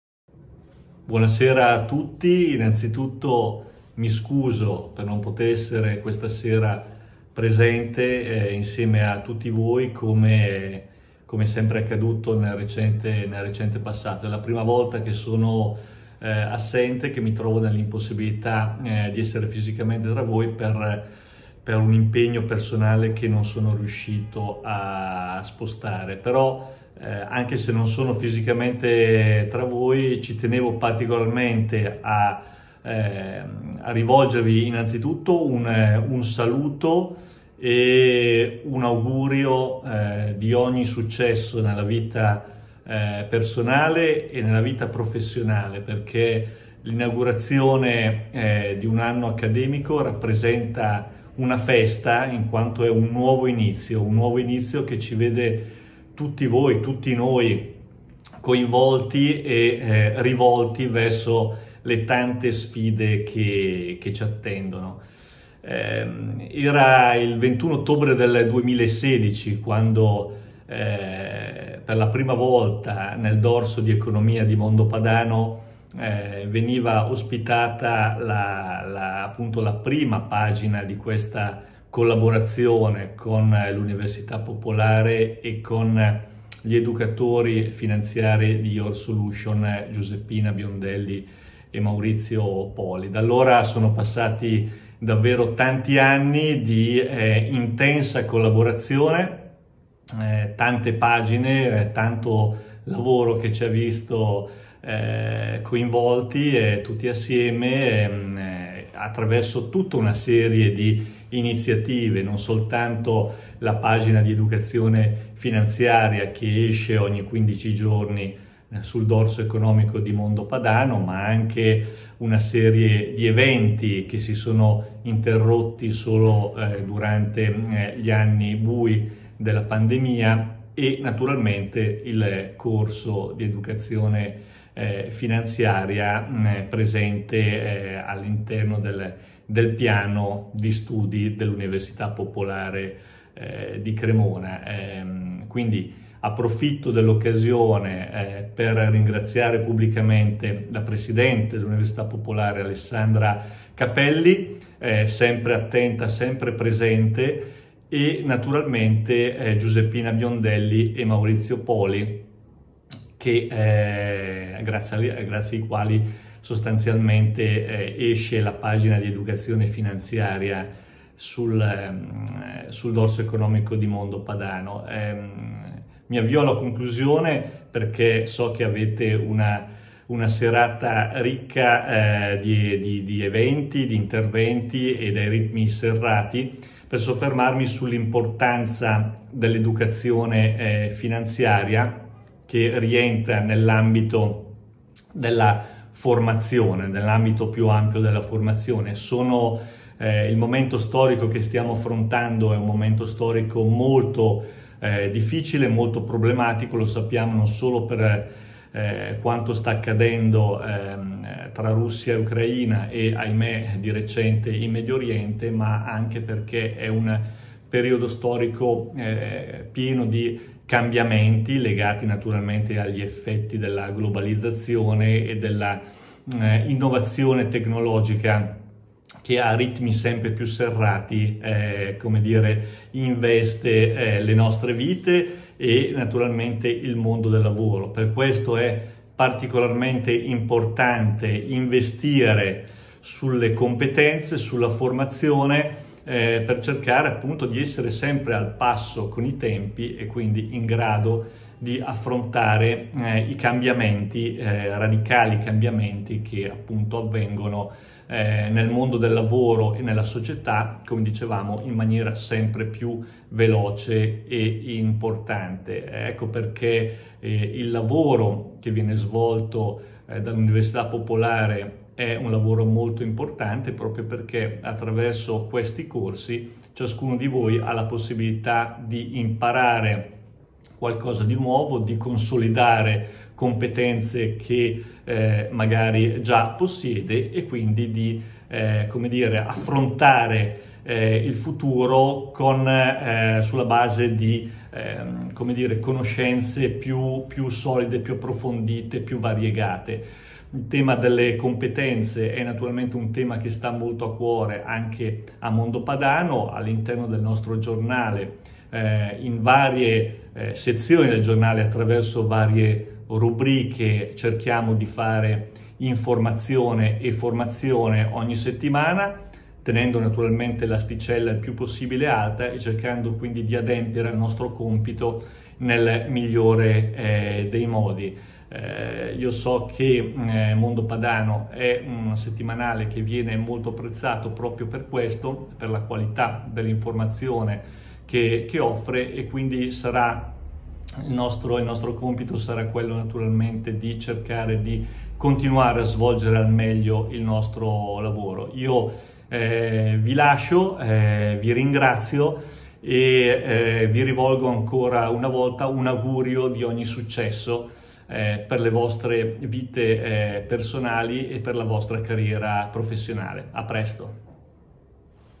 OTTOBRE 2023 MESE DELL'EDUCAZIONE FINANZIARIA saluto del Direttore di Mondo Padano Alessandro Rossi
«Oggi come ieri, il sapere rende liberi».
L’ Università Popolare di Cremona inaugura il nuovo anno accademico: docenti e corsi.
Presenti gli Educatori Finanziari Maurizio Poli e Giusy Biondelli

sull’Educazione Finanziaria.
ANCP - Associazione Nazionale Consulenti Patrimoniali EFPA Italia AIEF
YOUR SOLUTION SRL